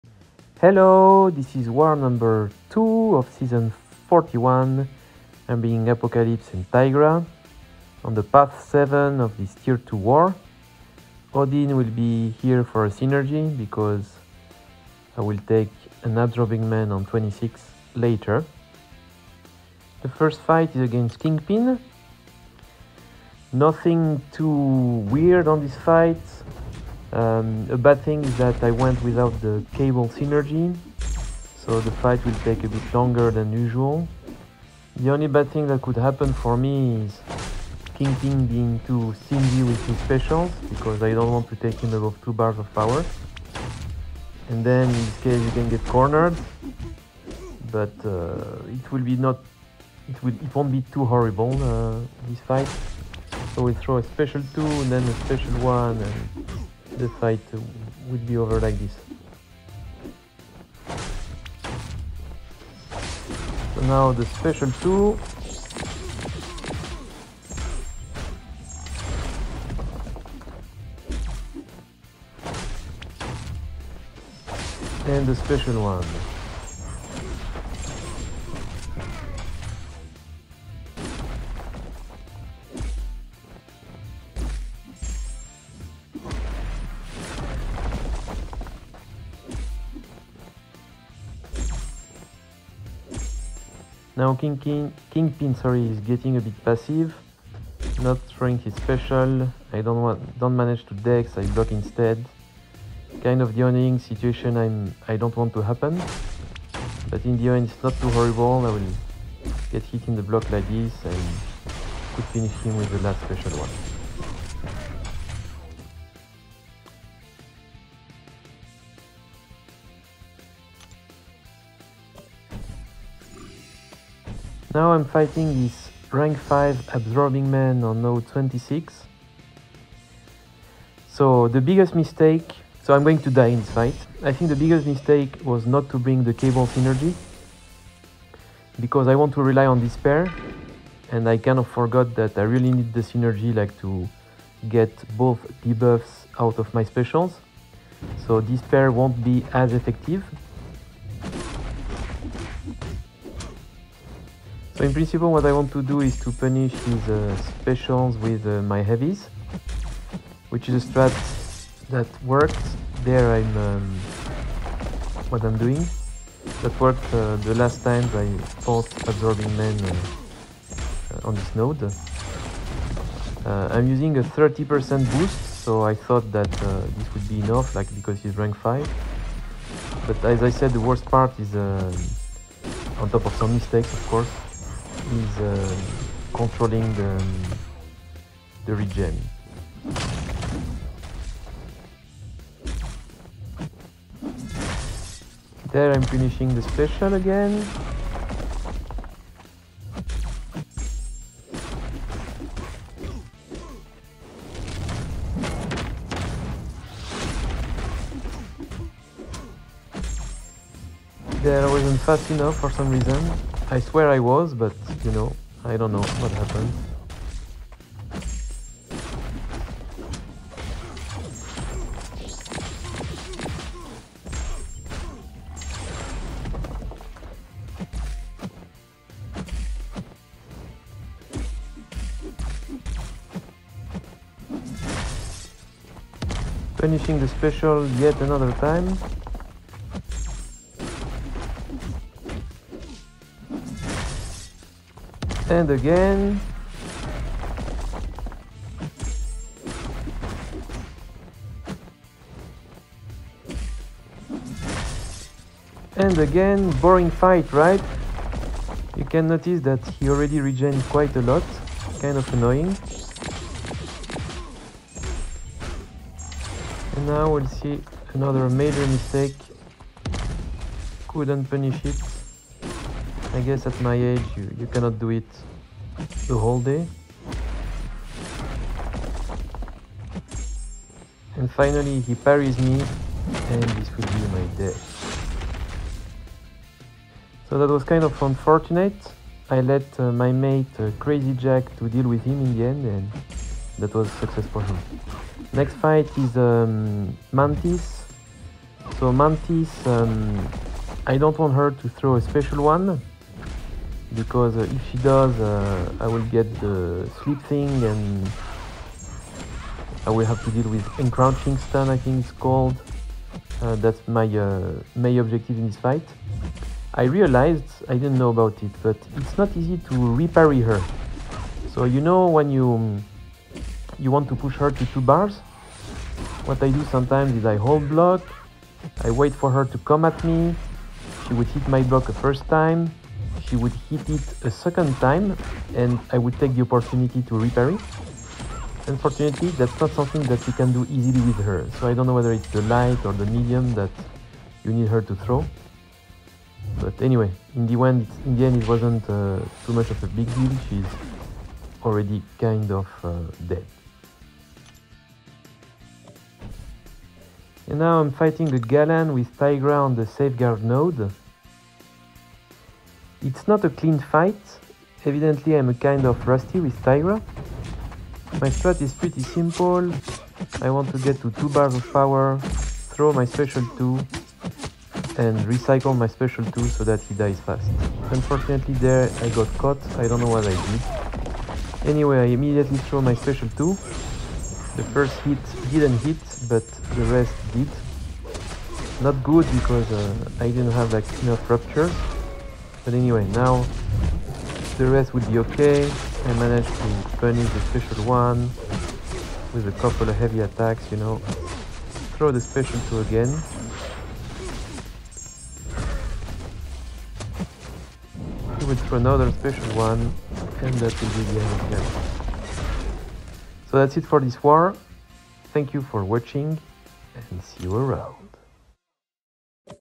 Season 41: war 2
I took Apocalypse and Tigra and the path 7 of this tier 2 planetary endurance war. The fights were:
- Kingpin vs Apocalypse
- Absorbing man vs Apocalypse (hazard shift/conflictor) - 1 DEATH 💀
- Mantis vs Apocalypse
- Galan vs Tigra (safeguard)